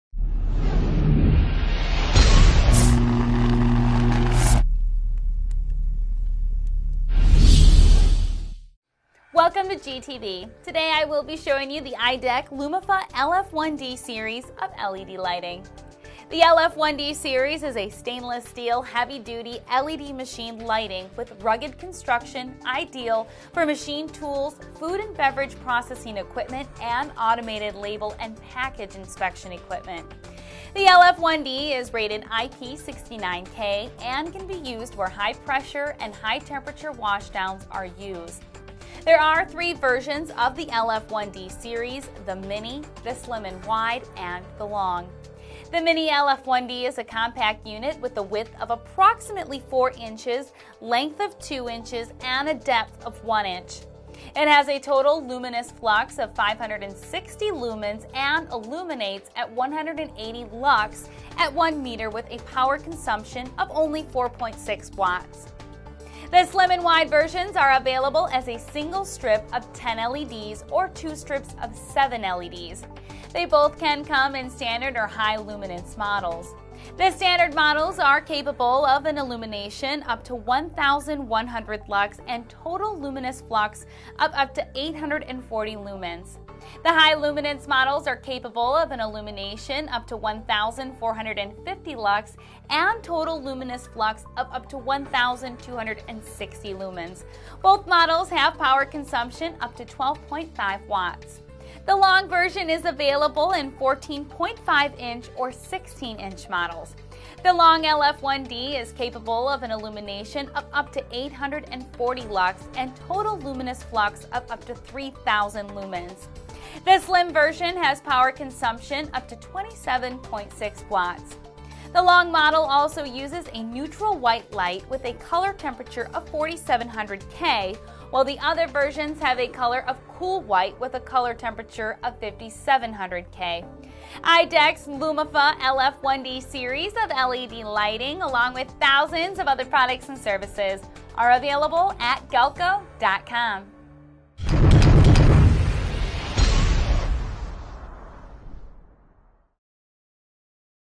IDEC Lumifa "LF1D" Series LED Lights - A GalcoTV Overview
IDEC Lumifa LF1D Series of LED Lighting presented by Katie Rydzewski for Galco TV.

IDEC's Lumifa LED Illumination Units (water, dust, and oil-proof). Available with or without cable gland, mounting bracket and cable. The slim model and wide model allows for multiple uses.

• Brightness:
-Slim: 66.6 Lumens/Watt
-Wide: 67.2 Lumens/Watt
• Life: 70% of initial luminance at
50,000 Hrs
• IP67 (polycarbonate lens) or IP67f
(reinforced glass lens)
• Available in clear or diffused Lens
• Surface mounting
• RoHS Compliant